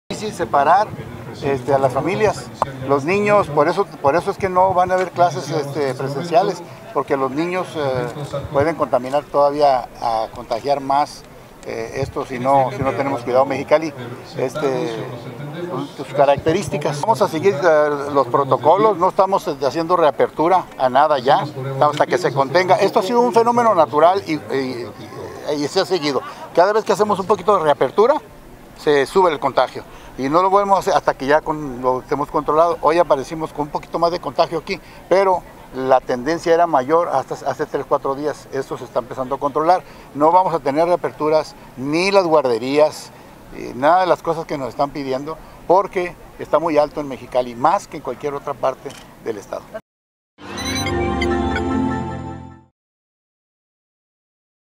Sin clases presenciales hasta que haya vacuna y cura: Jaime Bonilla
Abrir los centros educativos generaría un mayor número de contagios entre niños y docentes.